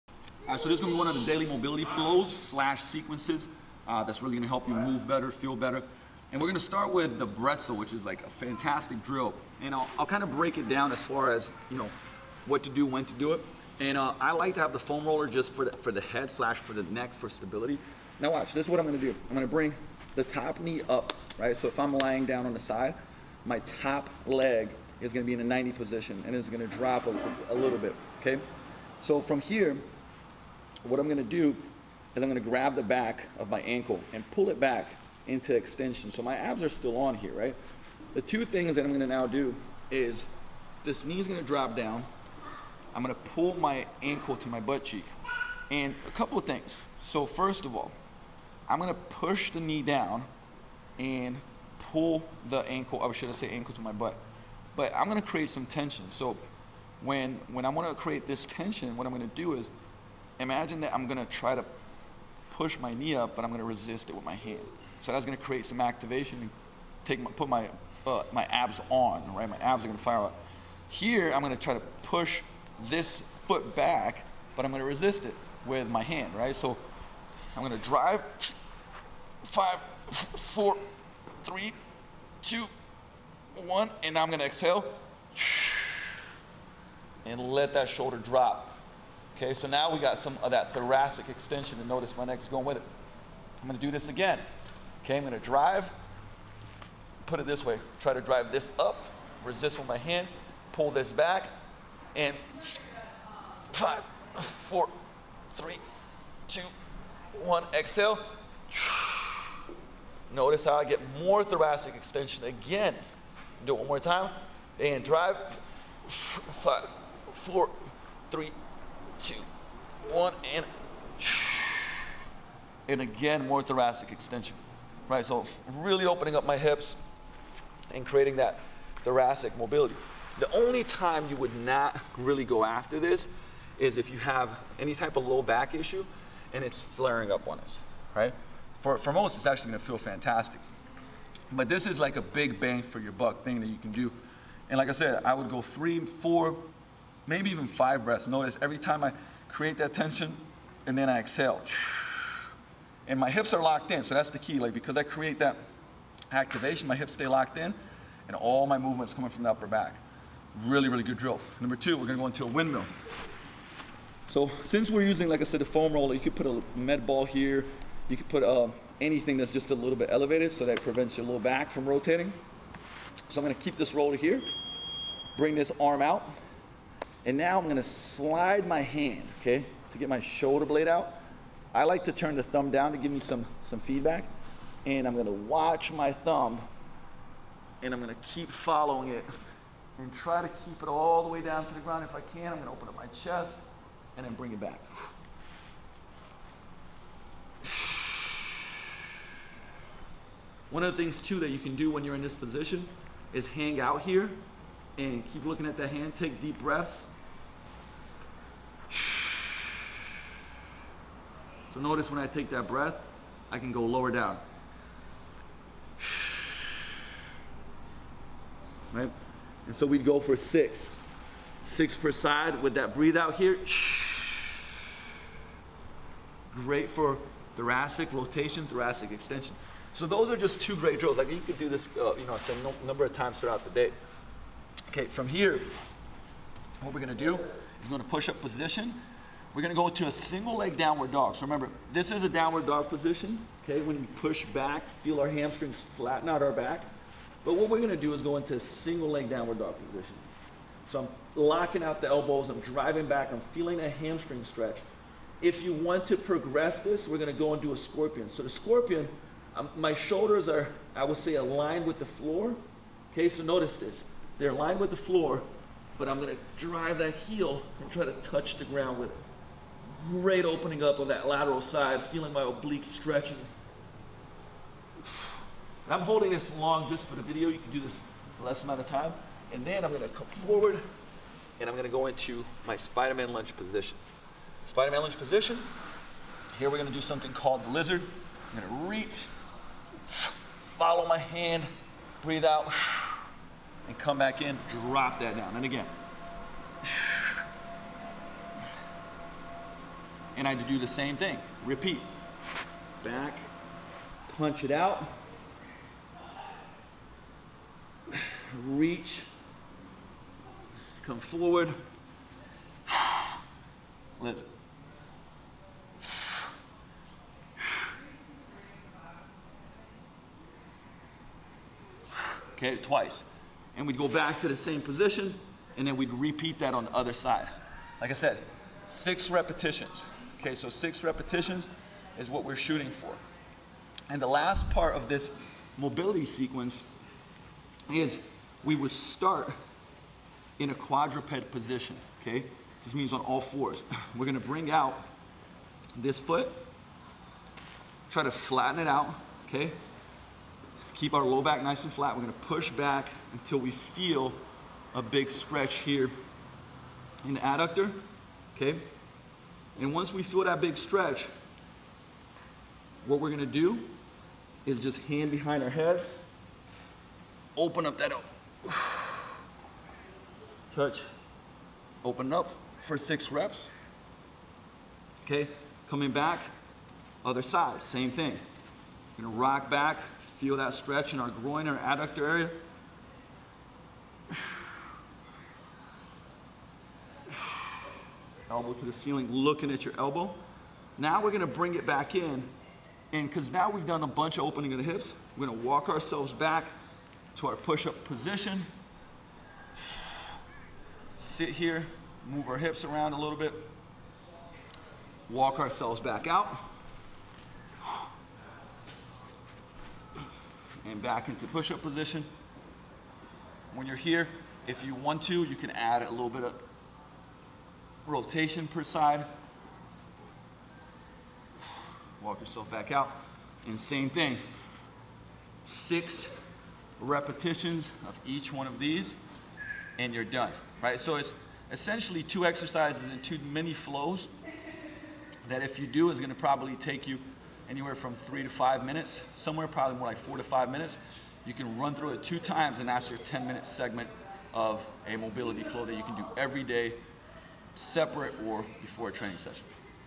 Do This Daily 10 Minute Mobility Sequence To Feel Great
Simplicity and consistency allow people to get the best results. With that said I wanted to share one of the daily mobility routines that are going to be a part of a 4 week kettlebell and bodyweight program.

I walk you through the sequence and show you what to do and you can put anywhere from 8 to 12 minutes on the timer and do this daily as a movement routine, a warm up, or both.

Let me know how you feel after.